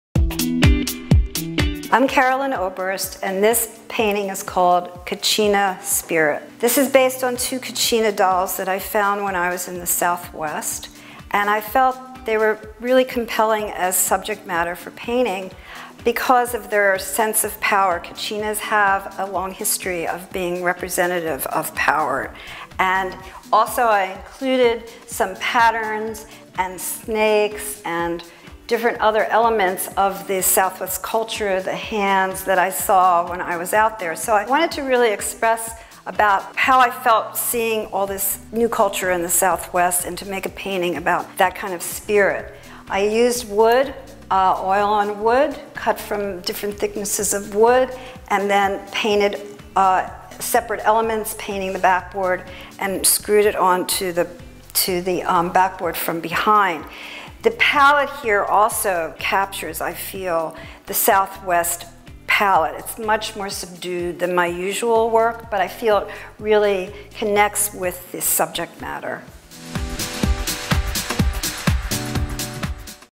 "Kachina Doll" Wood Relief Painting by Carolyn Oberst
Carolyn discusses the inspiration behind her Kachina Doll painting.